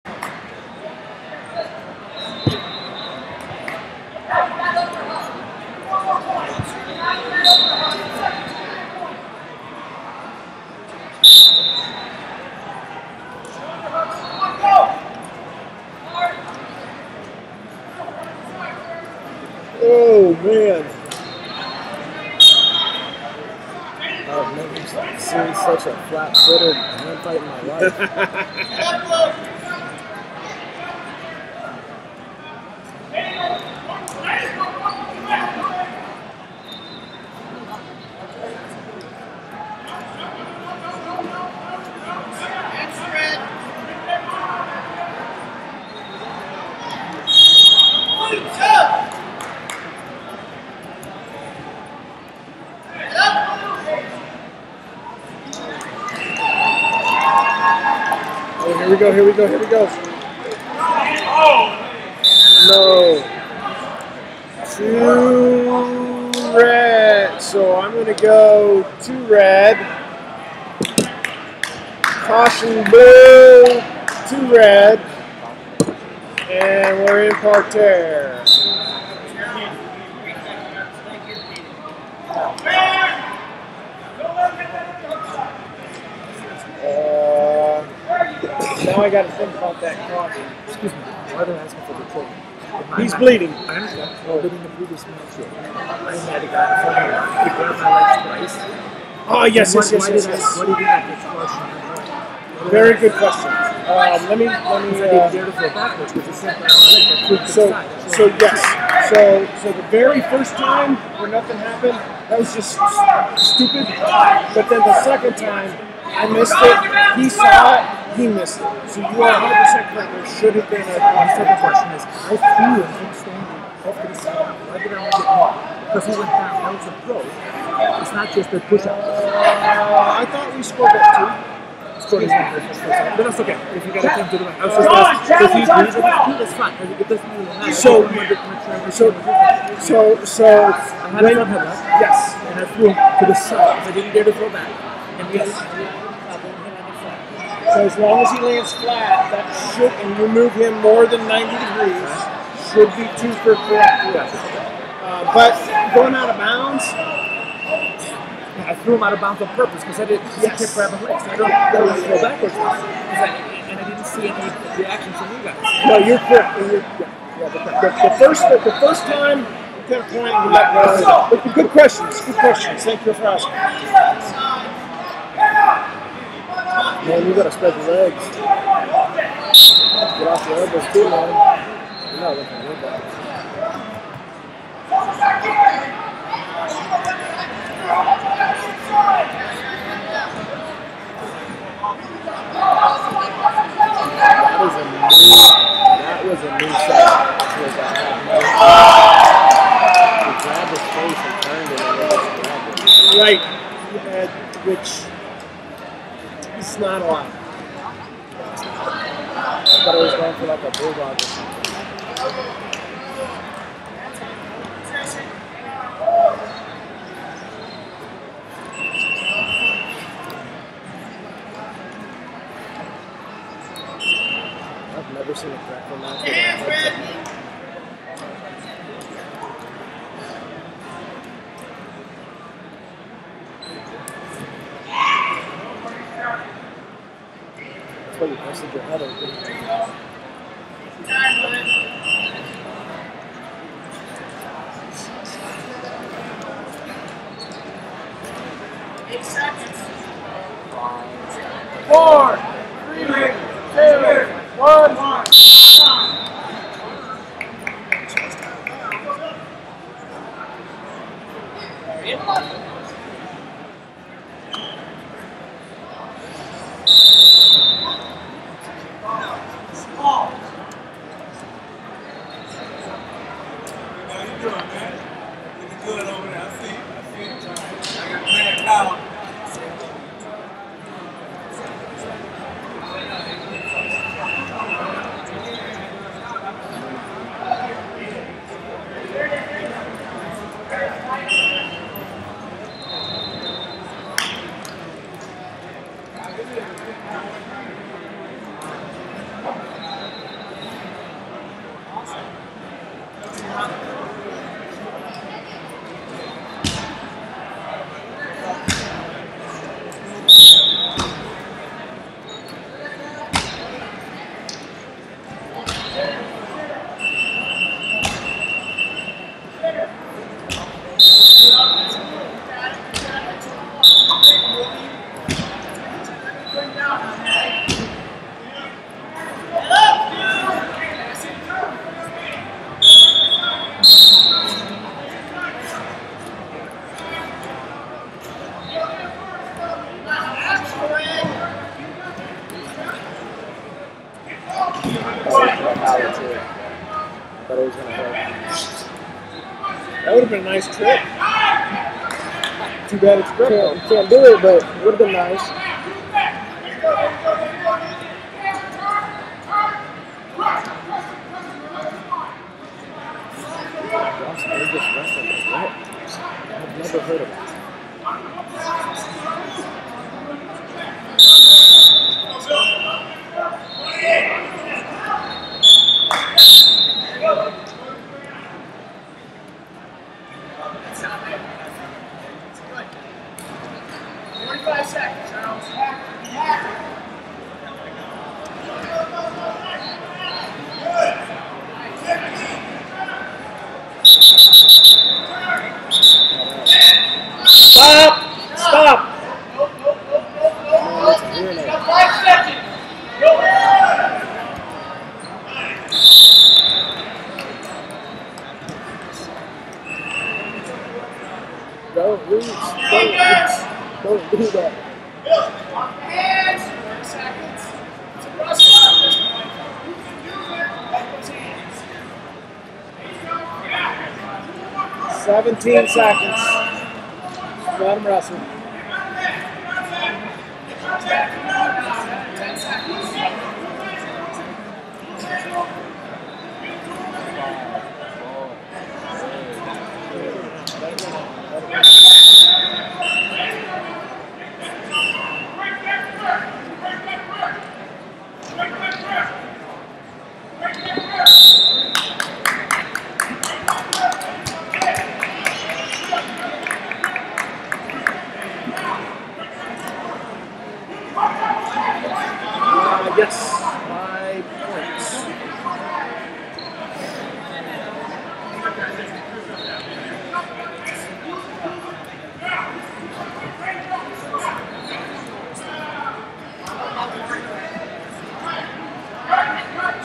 100 Kg Round 1 - Charles Jones, Las Vegas Wrestling Club Vs Joseph Cattan, Kentucky 30a5
Flosports-partner-access/2023 US Open Wrestling Championships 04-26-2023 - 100 Kg Round 1 - Charles Jones, Las Vegas Wrestling Club Vs Joseph Cattan, Kentucky 30a5